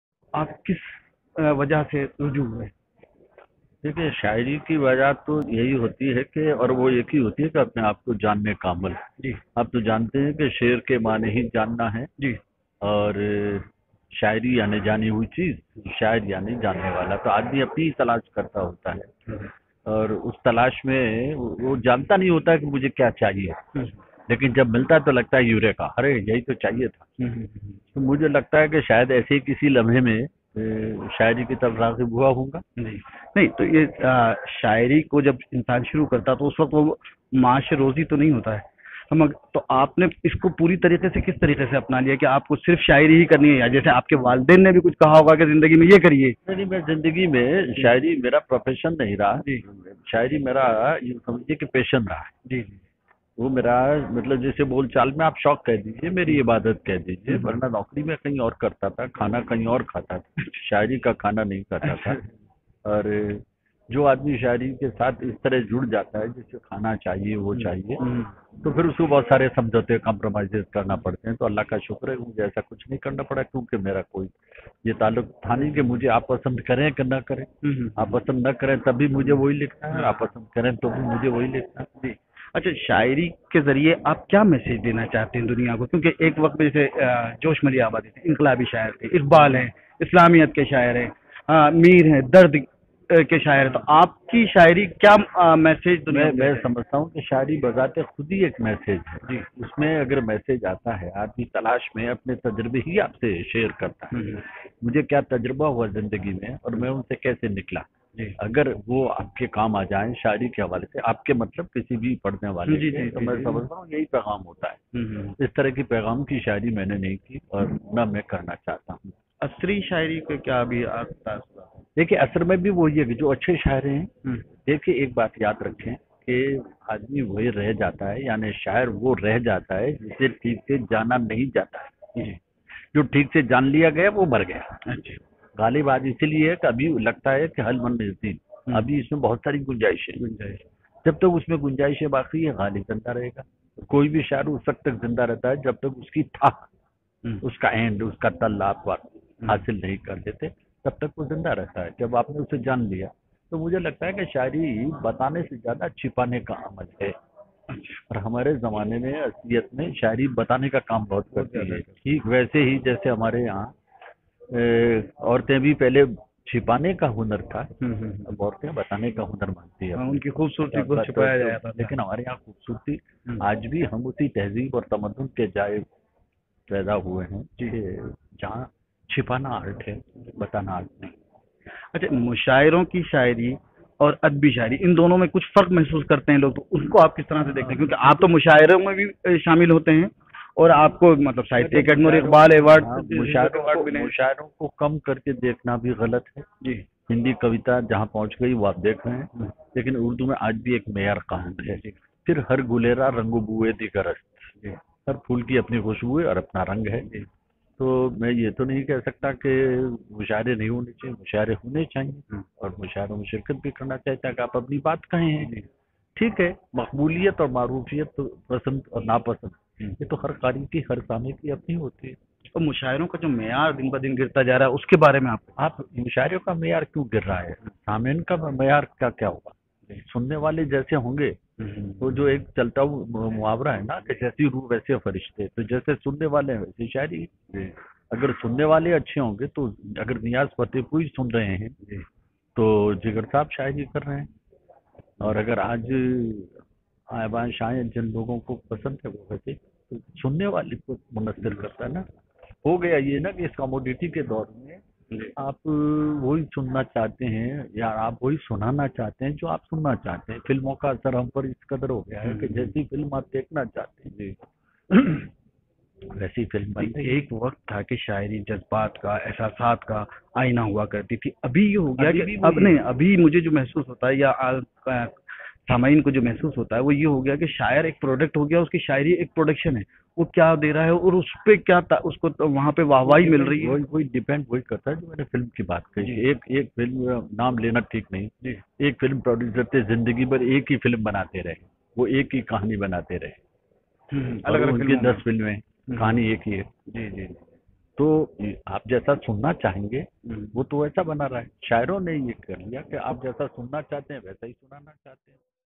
NN NEWS! SHEEN KAAFI NIZAAM SE GUFTUGU
VISHWAS RANG ME SHIRKAT KE  LIYE BHOPAL AAYE MASHHOOR AUR MARUF SHAYAR SHEEN KAAFI NIZAM SE NN NEWS NE KI GUFTAGU